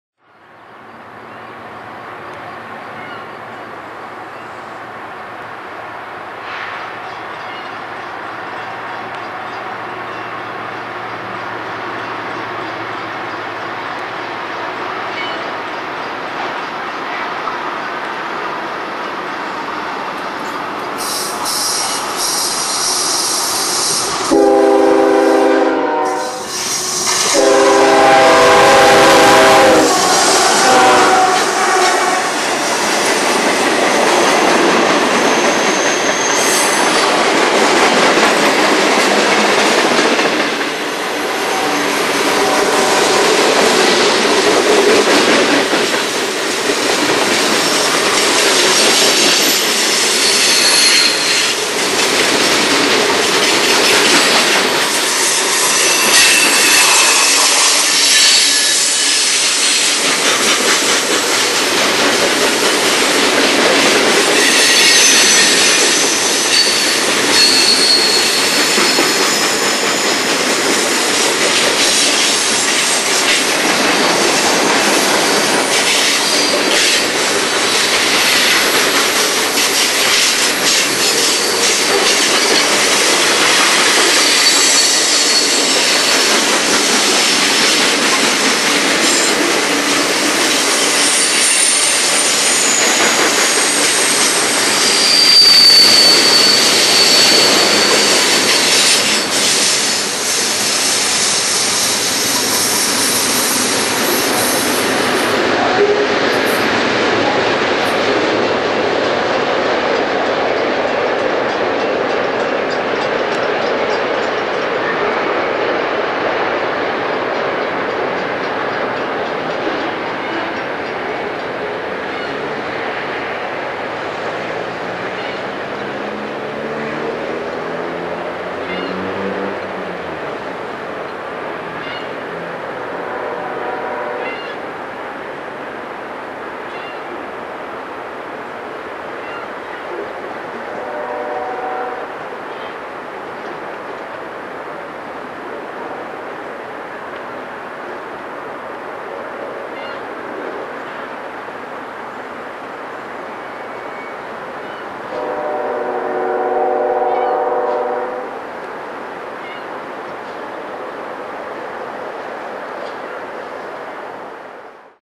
UP #8988 Leads The MRVSJ Through Fremont/Centerville
UP #8988 leads the M(Manifest) RV (Rosevile, CA) to SJ (San Jose, CA) while passing the Centerville/Fremont train depot.  Enjoy!!  Shot 5/7/14